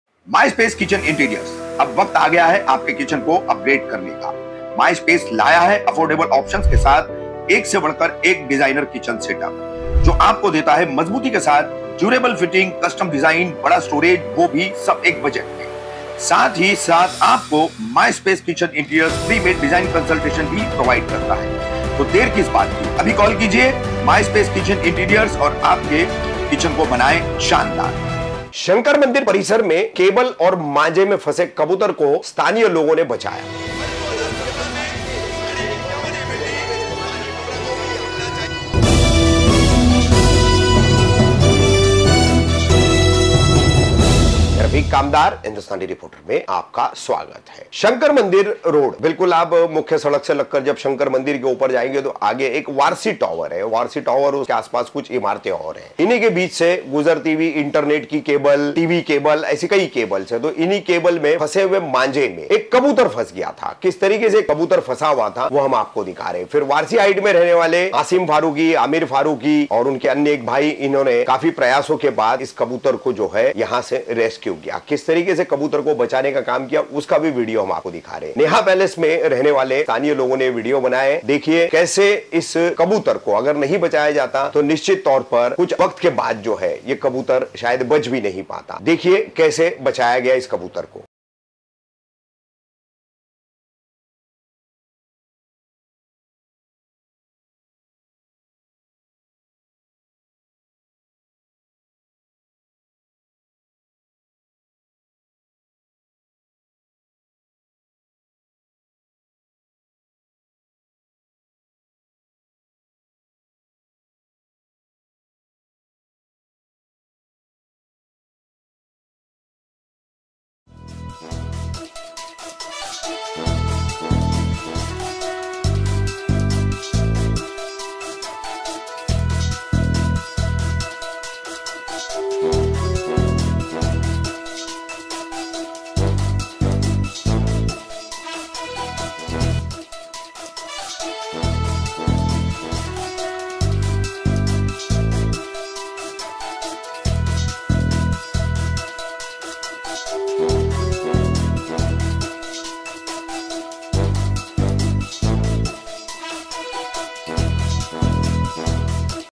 शंकरमंदिर परिसर में केबल और मांजे में फंसे कबूतर को स्थानीय लोगों ने बचाया| Hindustani Reporter
शंकरमंदिर परिसर में केबल और मांजे में फंसे कबूतर को स्थानीय लोगों ने बचाया